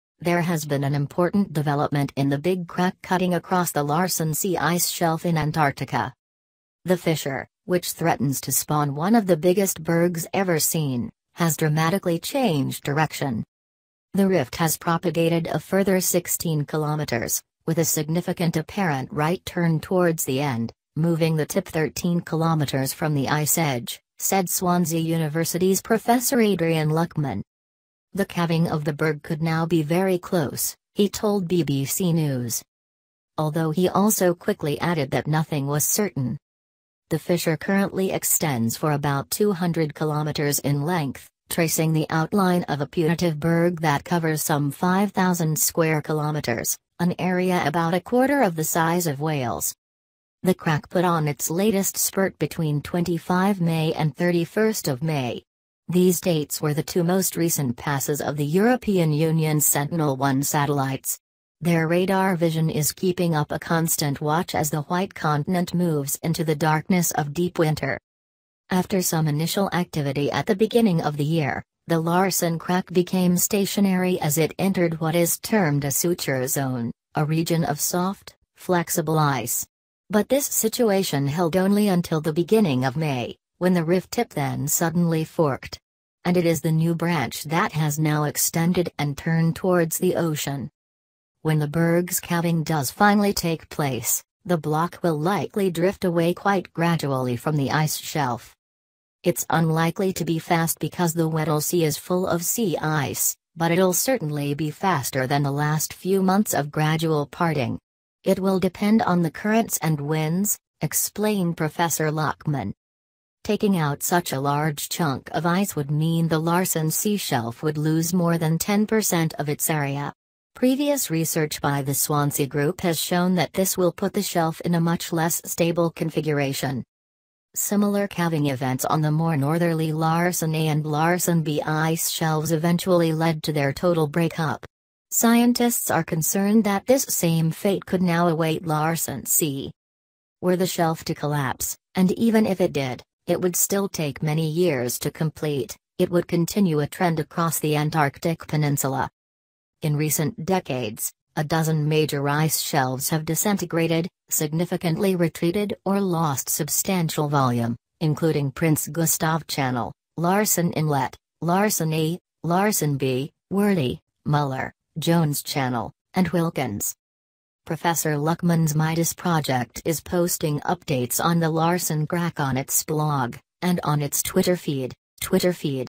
Antarctic ice crack takes major turn
There has been an important development in the big crack cutting across the Larsen C Ice Shelf in Antarctica.
The fissure, which threatens to spawn one of the biggest bergs ever seen, has dramatically changed direction.
"The rift has propagated a further 16km, with a significant apparent right turn towards the end, moving the tip 13km from the ice edge," said Swansea University's Prof Adrian Luckman.
The calving of the berg could now be very close, he told BBC News.
Although he also quickly added that nothing was certain.
The fissure currently extends for about 200km in length, tracing the outline of a putative berg that covers some 5,000 sq km - an area about a quarter of the size of Wales.
The crack put on its latest spurt between 25 May and 31 May. These dates were the two most recent passes of the European Union's Sentinel-1 satellites. Their radar vision is keeping up a constant watch as the White Continent moves into the darkness of deep winter.
After some initial activity at the beginning of the year, the Larsen crack became stationary as it entered what is termed a "suture" zone - a region of soft, flexible ice. But this situation held only until the beginning of May, when the rift tip then suddenly forked. And it is the new branch that has now extended and turned towards the ocean.When the berg's calving does finally take place, the block will likely drift away quite gradually from the ice shelf.
"It's unlikely to be fast because the Weddell Sea is full of sea-ice, but it'll certainly be faster than the last few months of gradual parting. It will depend on the currents and winds," explained Prof Luckman.
Taking out such a large chunk of ice would mean the Larsen C shelf would lose more than 10% of its area. Previous research by the Swansea group has shown that this will put the shelf in a much less stable configuration.
Similar calving events on the more northerly Larsen A and Larsen B ice shelves eventually led to their total break-up. Scientists are concerned that this same fate could now await Larsen C.
Were the shelf to collapse (and even if it did, it would still take many years to complete), it would continue a trend across the Antarctic Peninsula.
In recent decades, a dozen major ice shelves have disintegrated, significantly retreated or lost substantial volume - including Prince Gustav Channel, Larsen Inlet, Larsen A, Larsen B, Wordie, Muller, Jones Channel, and Wilkins.
Prof Luckman's MIDAS Project is posting updates on the Larsen crack on its blog, and on its Twitter feed.